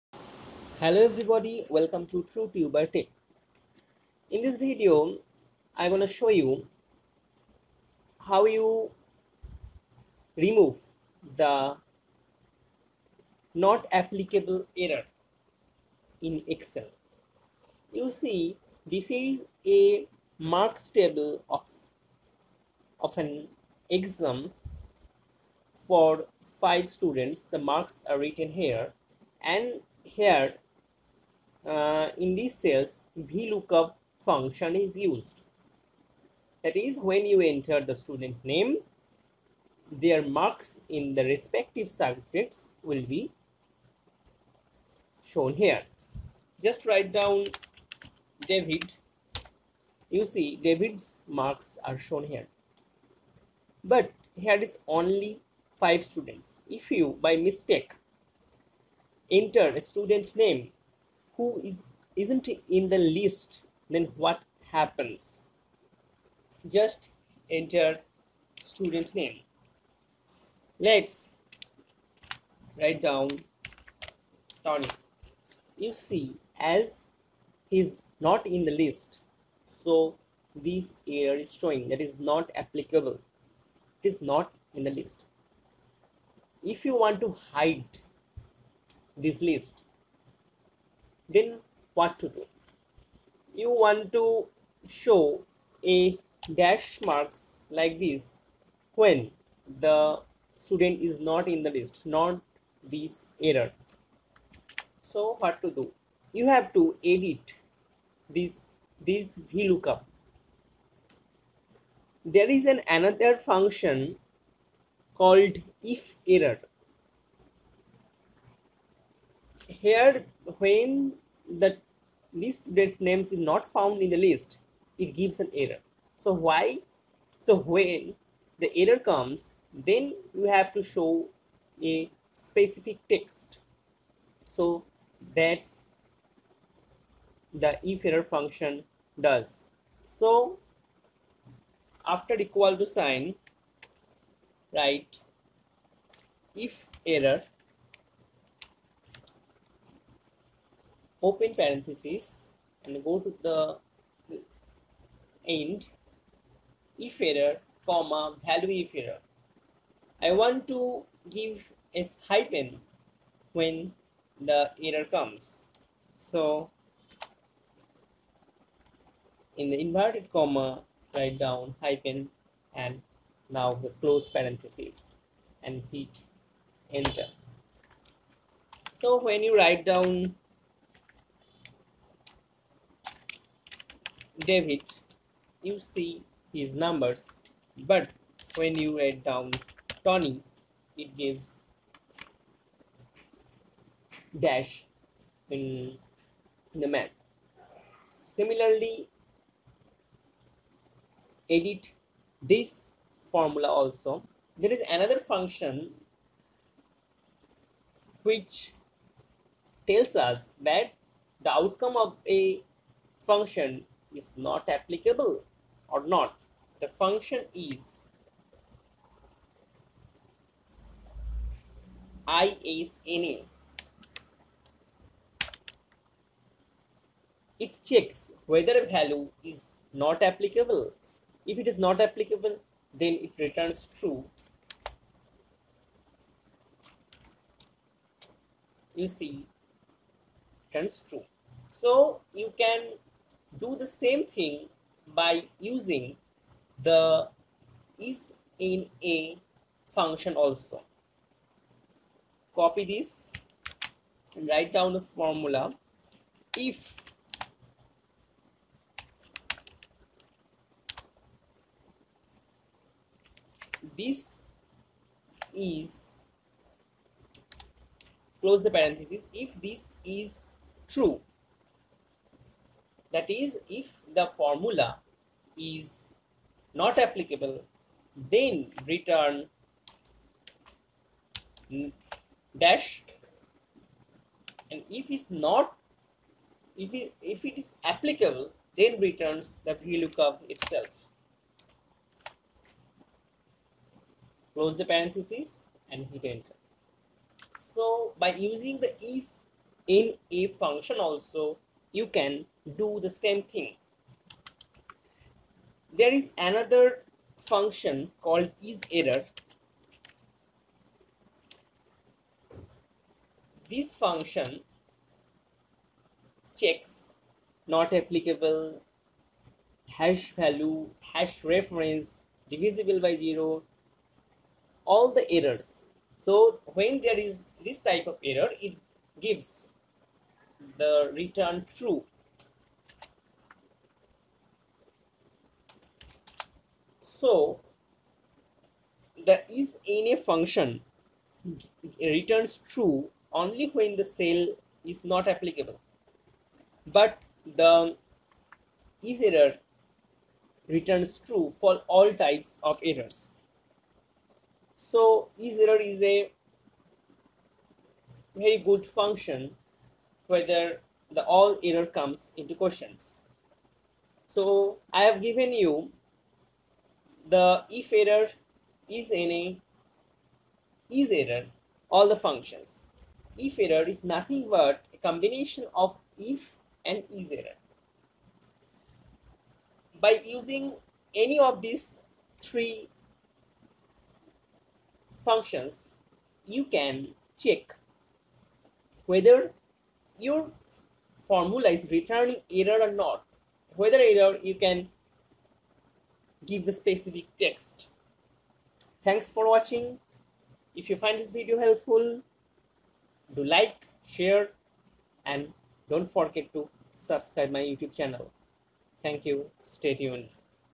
Remove the N/A Error from VLOOKUP in Excel
This video shows how to remove N/A Error in exvel.